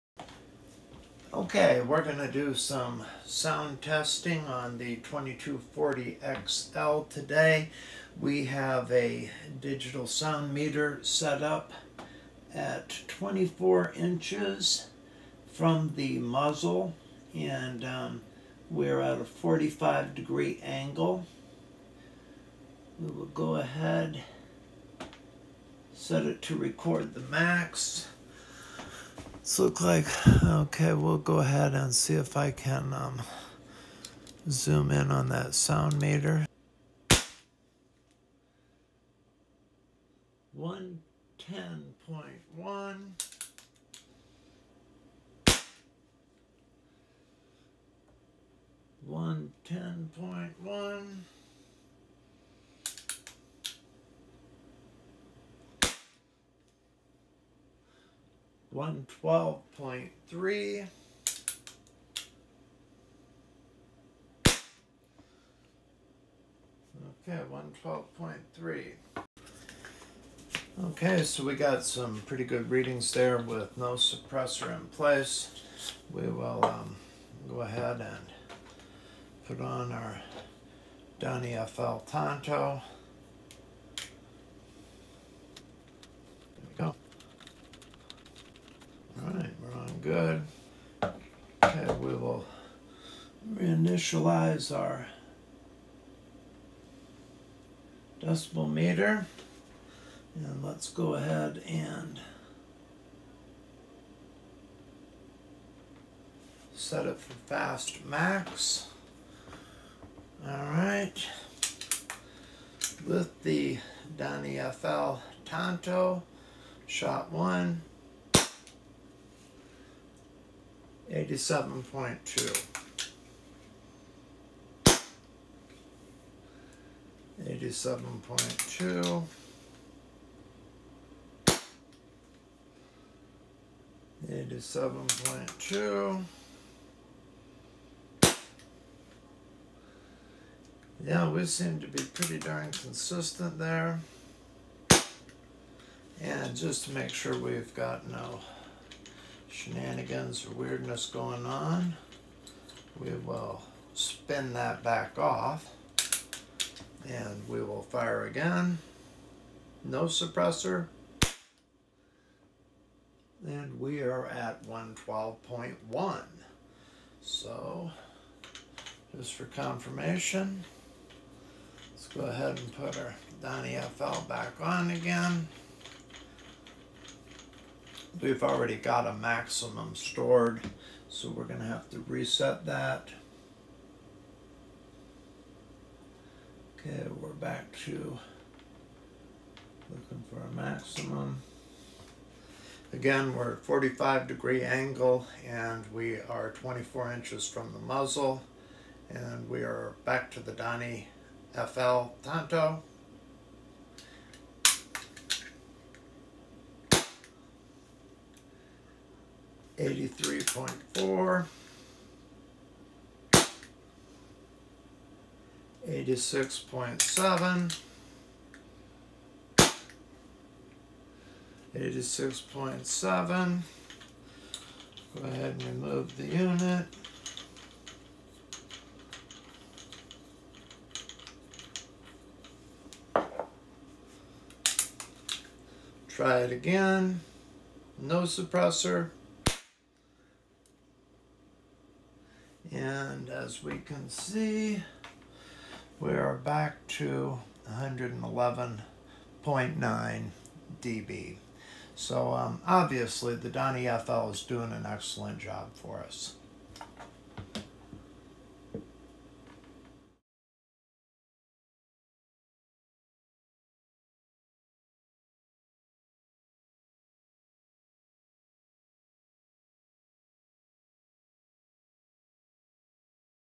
Sound Testing 2240XL & DonnyFL Tanto
In this episode we use a digital sound meter to test noise levels of the Crosman 2240XL with and without a DonnyFL Tanto model suppressor.

Or send us a SuperThanks through YouTube.

Not in a position to support financially ? No worries! Your likes, comments, subscription & views all help.

Shoot straight & be well my friend.